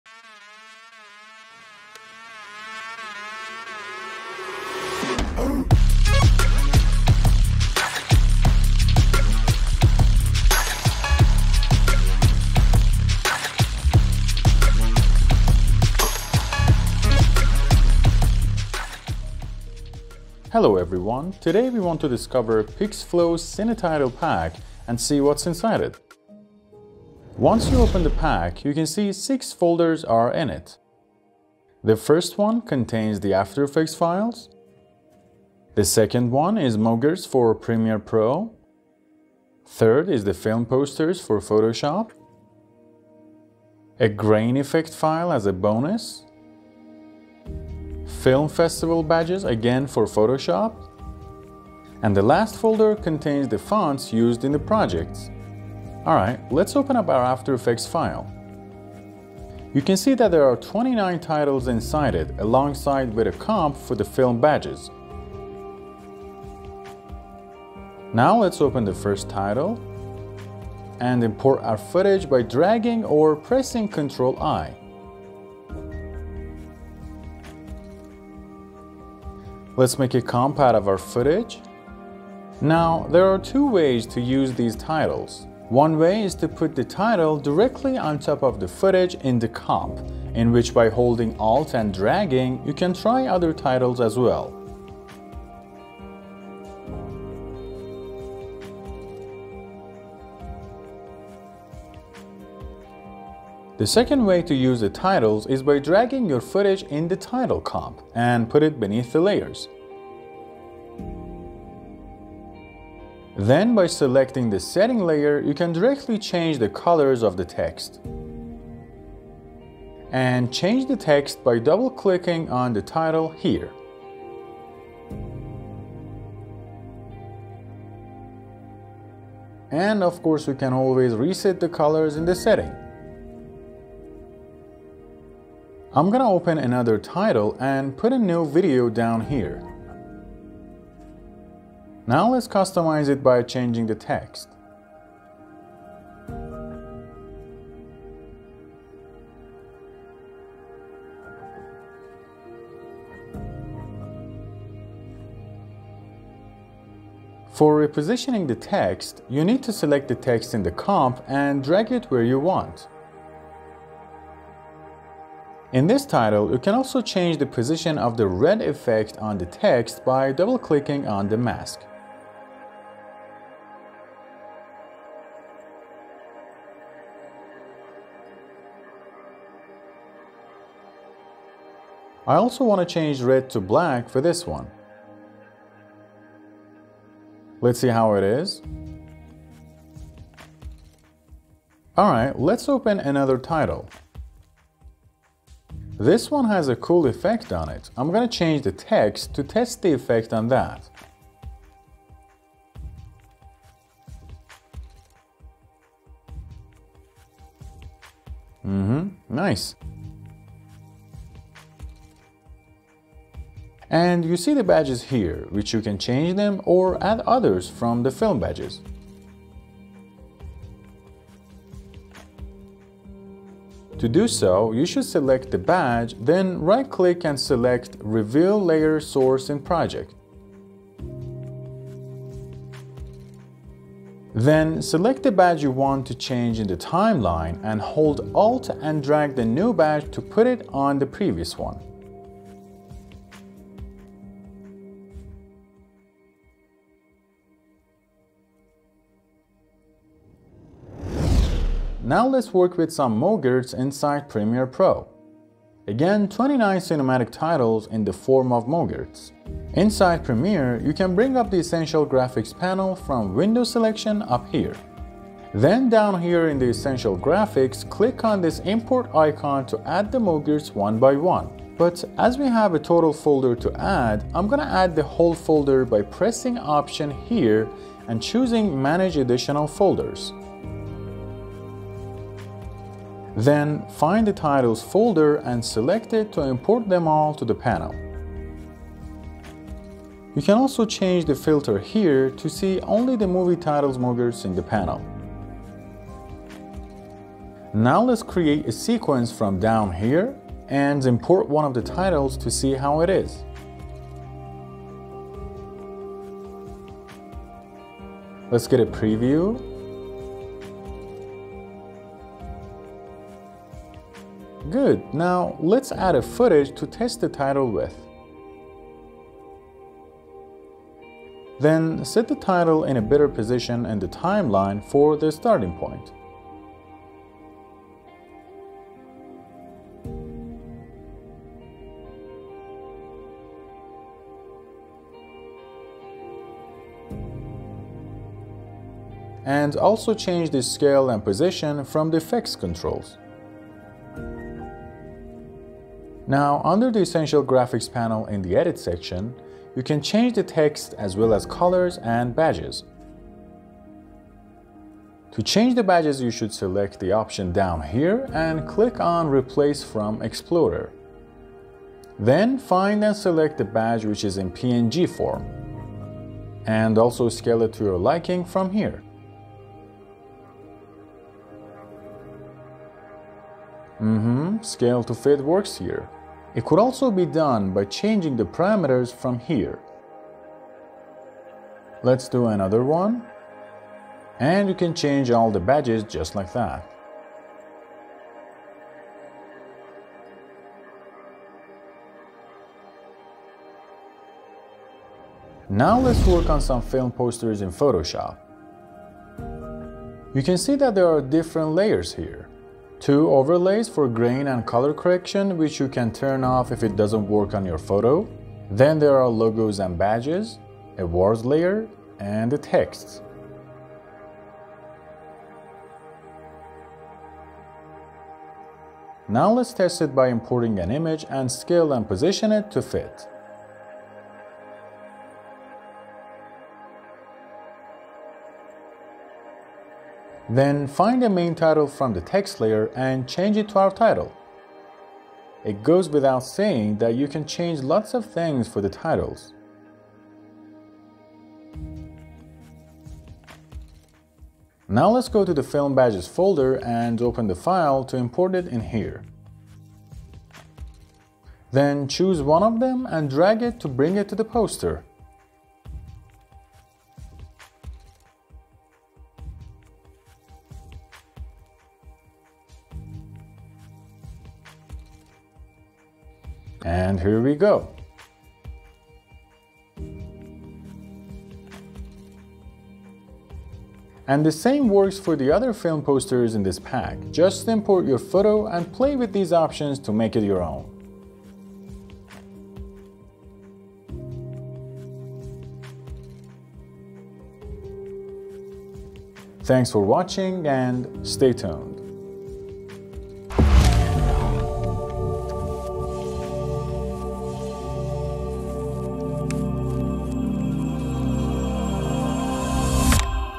Tutorial - How To Use CineTitle Pack In Ae, Pr, & Psd
Check out the in-depth tutorial on the best ways to make use of the CineTitle packs cinematic titles on your following projects in After Effects, Premiere Pro, and Adobe Photoshop.
29 cinematic titles and templates - designed to suit any cinematic project. Level up your next project now!

🎅🎁 Holiday Mega Sale
This is your one shot to stack your 2026 projects with every template, effect, and tool we’ve got.